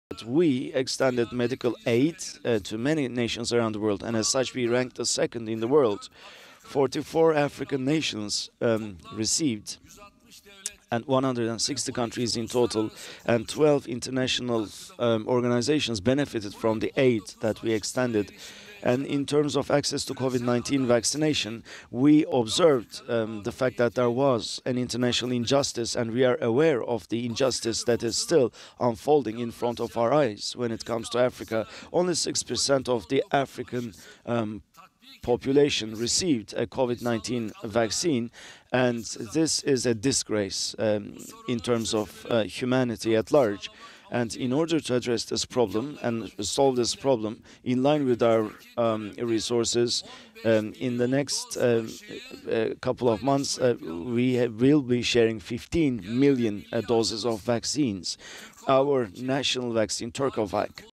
President Erdogan at the Third Turkiye-Africa Summit
Turkiye's President Erdogan addresses Turkiye-Africa Summit:

- Vaccine inequality and injustice towards Africa is a disgrace and must stop
- 15m doses of our TurkoVac will first be shared with Africa
- We'll also continue supplying medical aid and equipment